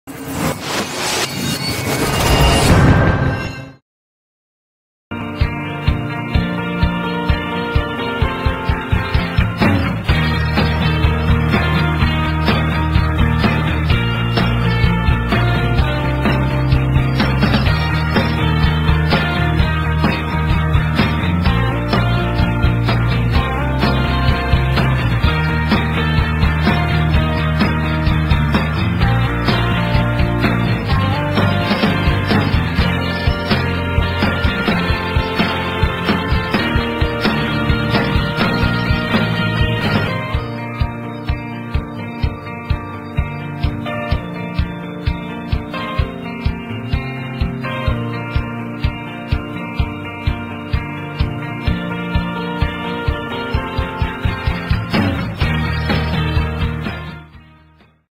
2 Color Balloon Automatic Screen Printing Machine
This kind screen printing machine is specially designed for printing the balloons,it can complete 2 colors printing at one time. easy to install and operate,and the max.printing speed can be up to 1200PCS/Hr,the printing area is 150*150mm.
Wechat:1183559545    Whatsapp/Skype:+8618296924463